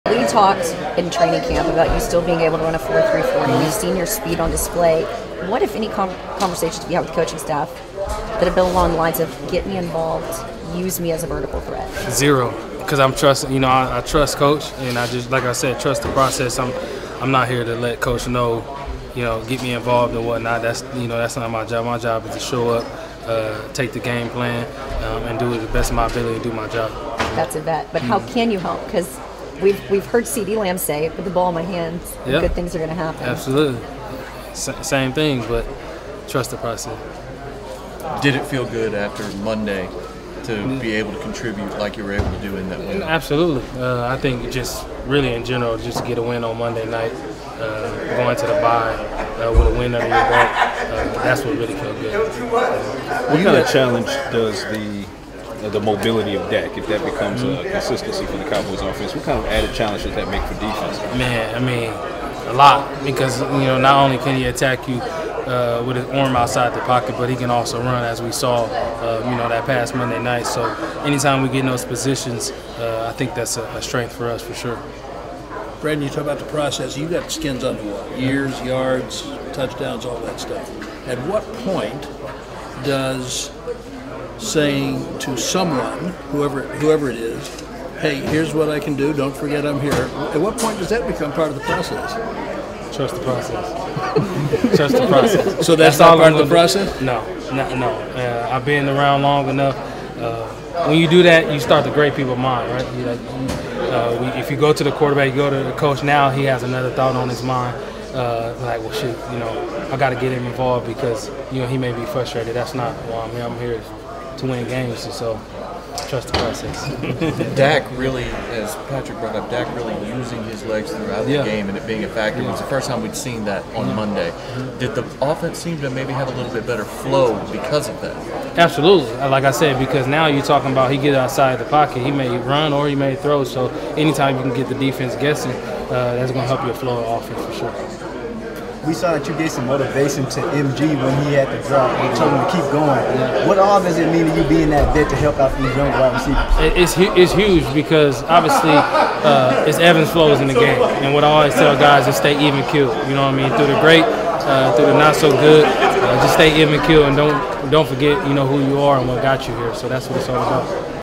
Brandin Cooks: Trust the Process | LARvsDAL | Dallas Cowboys 2023
Wide receiver Brandin Cooks talks about trusting the process the coaching staff has in place, his role in the Cowboys offense and more.